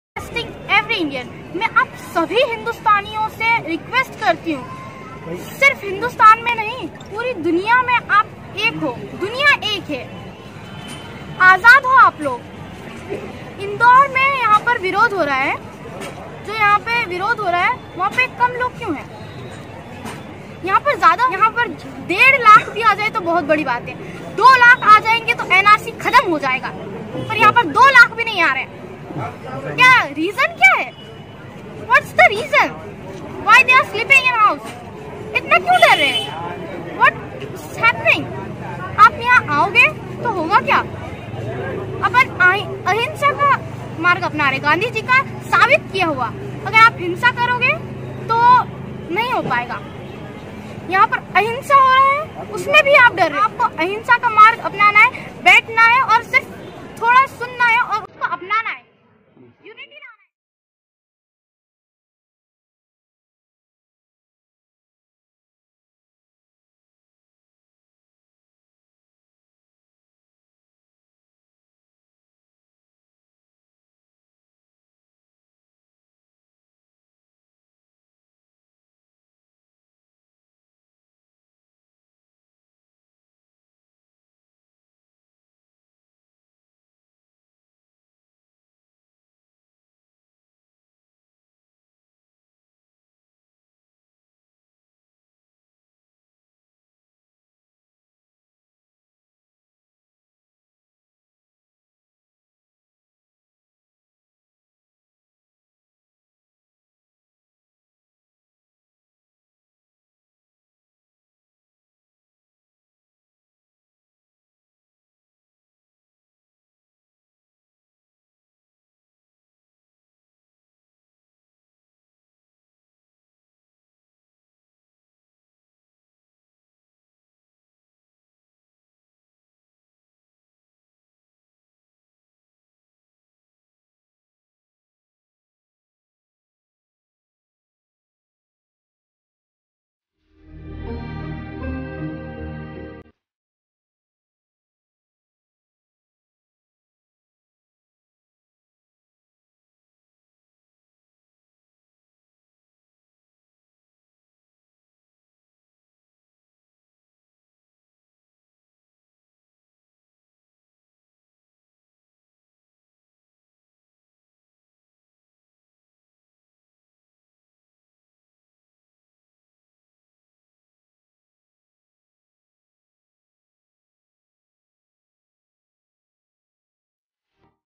छोटे बच्चों को बनाया जा रहा हैं मोहरा। इंदौर के शाहीन बाग बडवाली चौकी पर CAA के विरोध में प्रदर्शन
इंदौर की बड़वाली चौकी में caa के खिलाफ विरोध प्रदर्शन में नन्हे नन्हे बच्चों को सरकार के खिलाफ भड़काया जा रहा हैं।